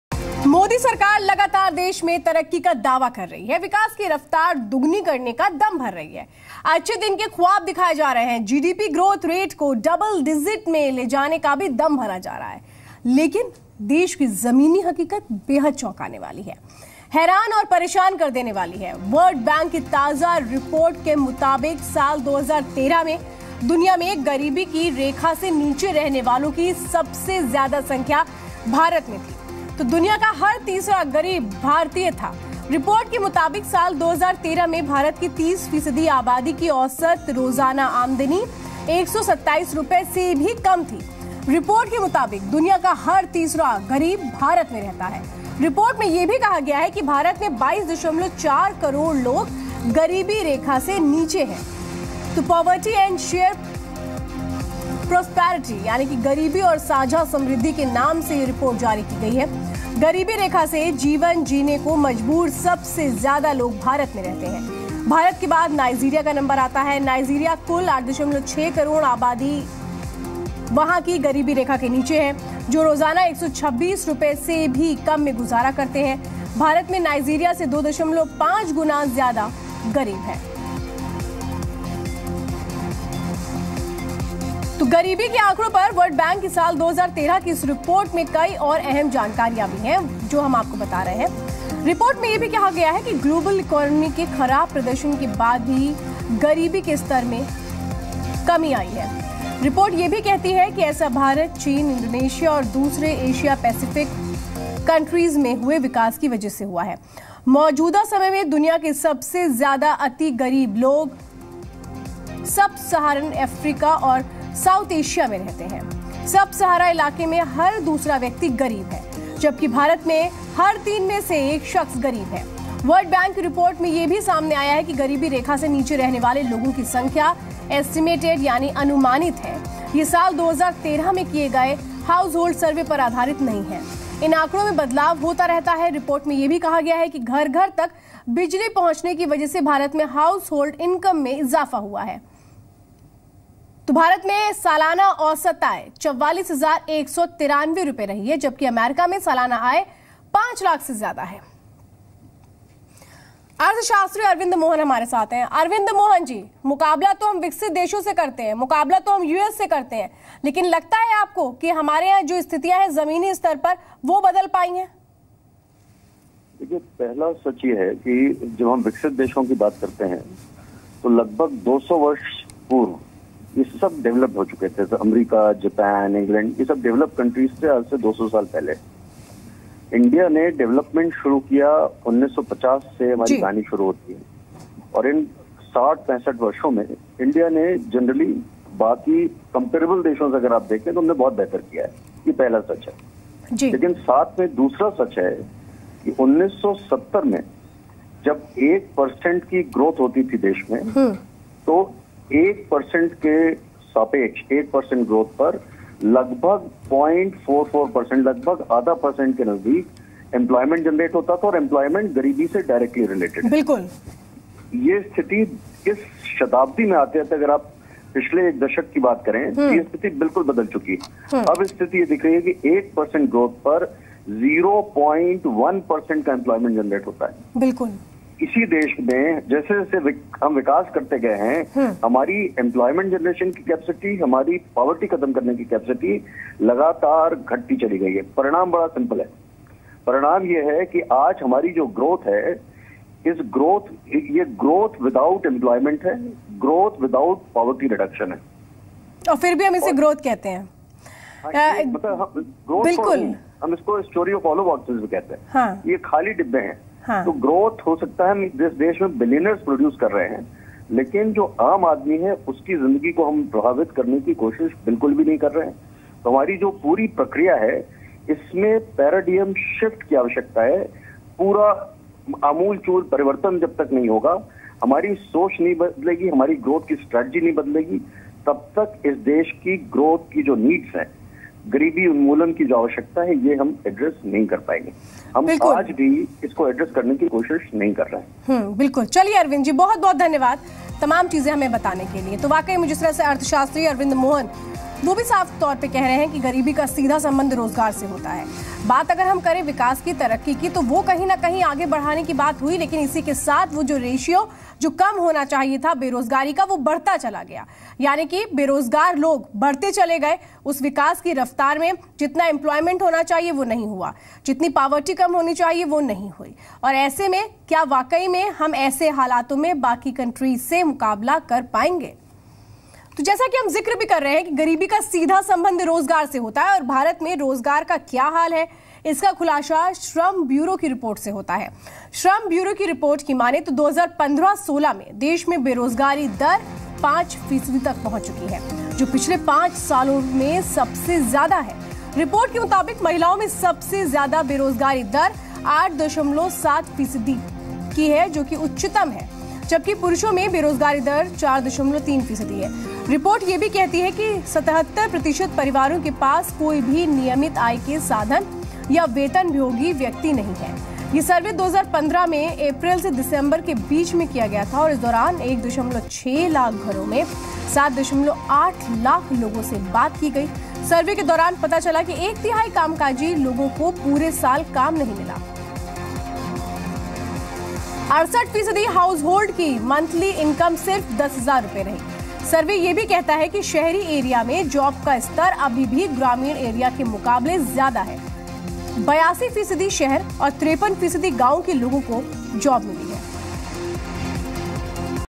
World Bank report on the poverty shows shocking position of India
World Bank report on the poverty shows the shocking position of India. Watch this special segment and get to know more here.